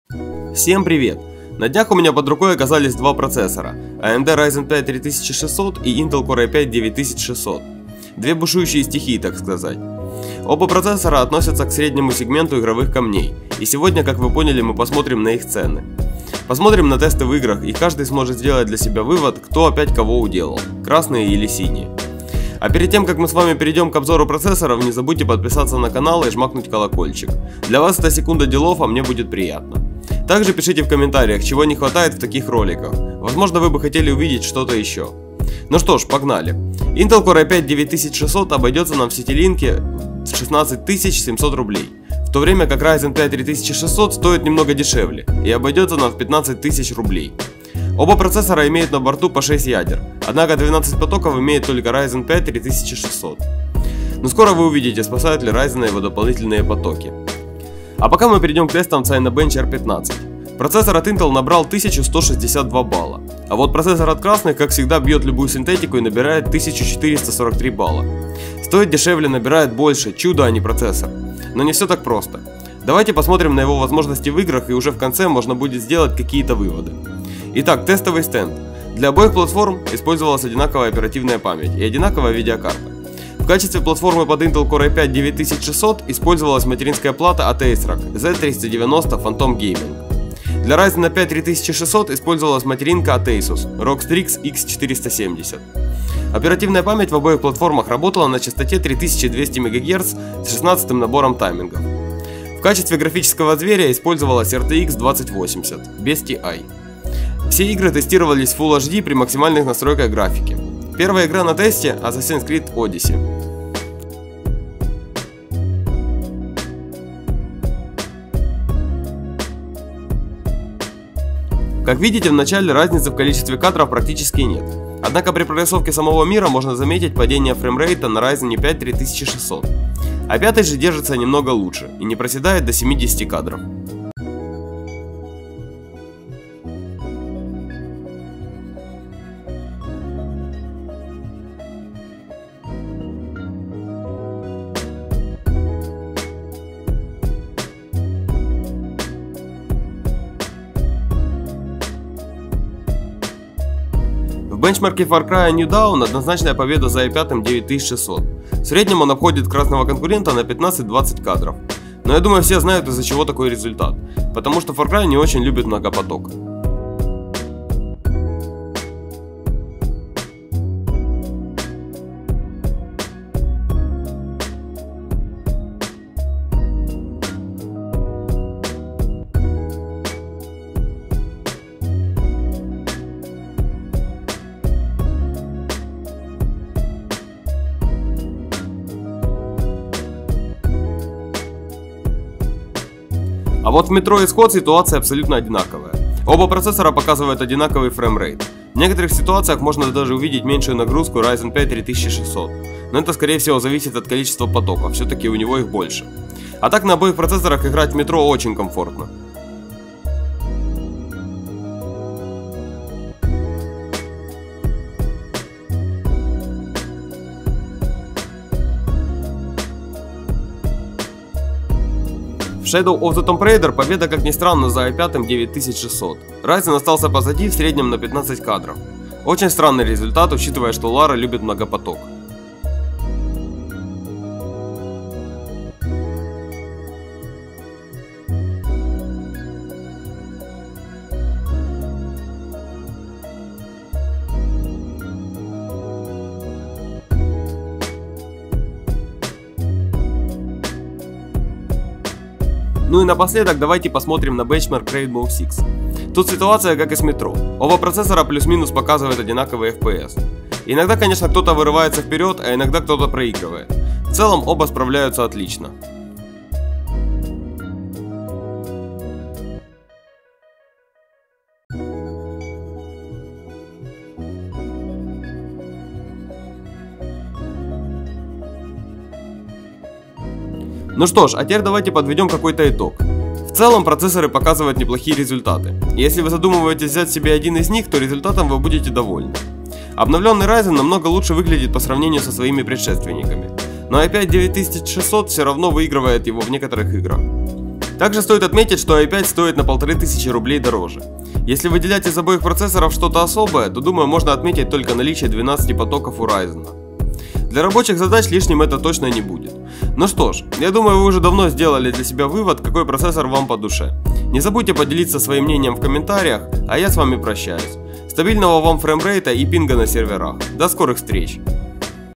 AMD Ryzen 5 3600 или Core i5 9600 - ищем топ за свои деньги
AMD Ryzen 5 3600 и Intel Core i5 9600 - лучшие процессоры бюджетного и среднего игрового ПК. Какой же из них лучше? Давайте разбираться. AMD Ryzen 5 3600 имеет 6 ядер и 12 потоков.  Основан на архитектуре Zen 2 от процессорного гиганта AMD. Райзен 5 3600 имеет базовую частоту 3,6 ГГц с бустом до 4,2 ГГц. Решение от АМД подходит как для игр, так и для рабочих задач (стрим, монтаж). Противоположный процессор от компании Intel - Core i5 9600 имеет 6 ядер и 6 потоков. Основан на архитектуре Coffe Lake Refresh. Имеет базовую частоту 3,7 ГГц с бустом до 4,6 ГГц. Интел Кор Ай 5 9600 подходит как для игр, так и для рабочих задач (стрим, монтаж). Однако процессор Intel имеет на борту 6 поток в отличии от AMD Ryzen 5 3600 с 12 потоками. Главная цель этого видео - сравнить AMD Ryzen 5 3600 и Intel Core i5 9600 и понять влияют ли дополнительные 6 потоков у АМД на фпс в играх. Тестировался Ryzen 5 3600 без разгона, в стоке. Intel Core i5 9600 так же был в стоке. Что Ryzen 5 3600, что Core i5 9600 в связке с RTX 2080 показали отличные результаты. Оба процессора способны показывать отличный фреймрейт и предоставлять комфортный гейминг. Так Intel или AMD спросите вы? Прямого ответа я вам не дам. Я лишь показал вам цену на Intel Core i5 9600 и AMD Ryzen 5 3600 на момент монтажа ролика. И показал вам тесты в играх и Cinebench R15. Имея на руках два процессора от AMD и Intel я могу уверенно сказать, что проигравших тут нет. Единственный момент - это зависимость Ryzen от частоты оперативной памяти. Однако Core i5 тоже не покажет хороший результат на 2133 МГц. Одно ясно точно, что в 2019 году развернулась отличная баталия между AMD и Intel. А конкуренция всегда приводит к снижению цен, что на руку всем нам.